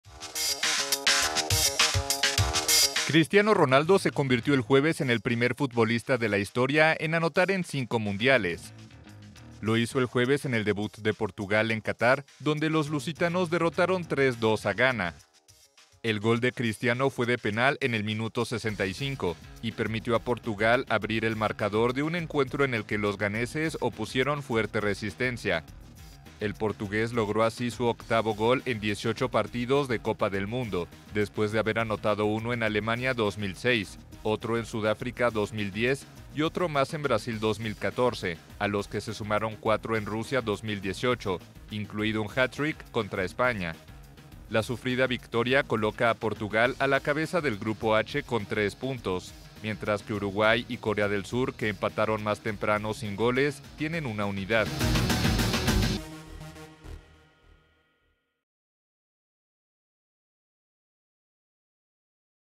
Cristiano Ronaldo, primer jugador en marcar en cinco Mundiales | AFP
Portugal logró el jueves una sufrida victoria ante Ghana por 3-2, en partido del Grupo H del Mundial en el que Cristiano Ronaldo se convirtió en el primer futbolista de la historia en anotar en cinco Mundiales.